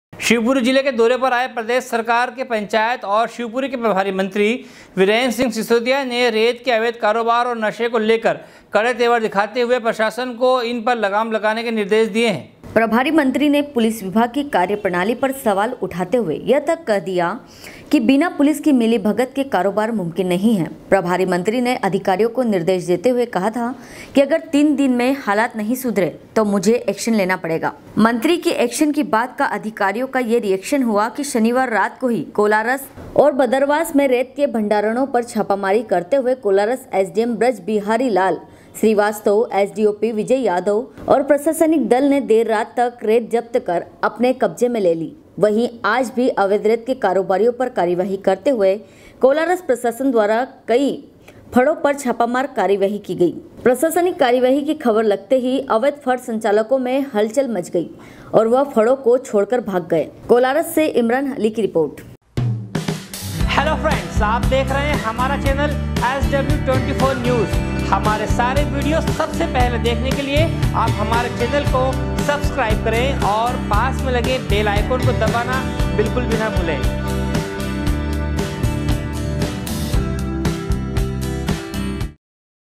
अवैध कारोबार और नशे को लेकर कड़े तेवर दिखाते हुए प्रशासन को इन पर लगाम लगाने के निर्देश दिये थे।
शिवपुरी जिले के दौरे पर आए प्रदेश सरकार में पंचायत प्रभारी मंत्री महेन्द्र सिंह सिसौदिया ने रेत के अवैध कारोबार और नशे को लेकर कड़े तेवर दिखाते हुए प्रशासन को इन पर लगाम लगाने के निर्देश दिये थे। कोलारस से इमरान अली की रिपोर्ट